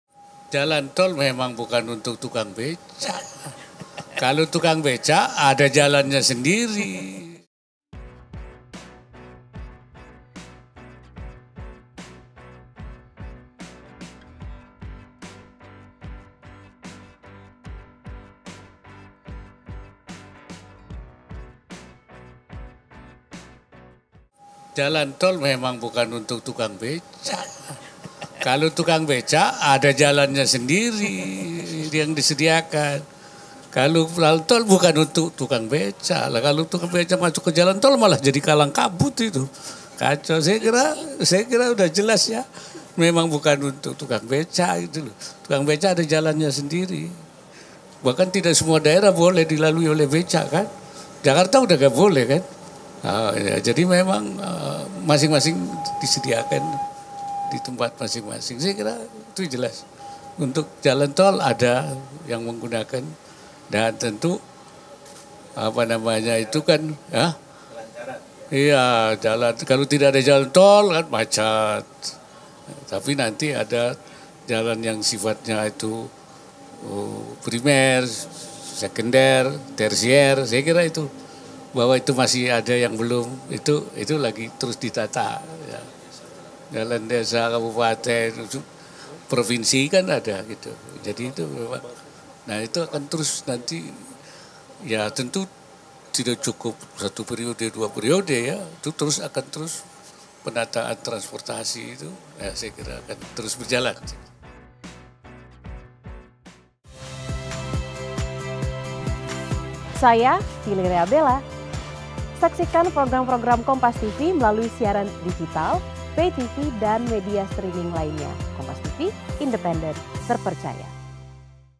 Wapres Ma'ruf Amin Tanggapi Kritik Cak Imin Soal Manfaat Jalan Tol Tak Dirasakan Tukang Becak
JAKARTA, KOMPAS.TV  Wakil Presiden Maruf Amin tanggapi soal kritikan dari calon wakil presiden nomor urut 1, Muhaimin Iskandar yang menyebut soal manfaat tol yang tidak dirasakan oleh tukang becak.

Hal ini disampaikan Maruf Amin saat ditemui di Istana Wapres pada Rabu (20/12/2023)

Menurut Maruf Amin jika tukang becak masuk jalan tol malah akan menjadi kalang kabut.

"Kalau urusan jalan tol memang bukan untuk tukang becak, kalau tukang becak ada jalannya sendiri yang disediakan," ujar Maruf Amin.

"Kalau tukang becak masuk ke jalan tol, malah jadi kalang kabut," lanjutnya.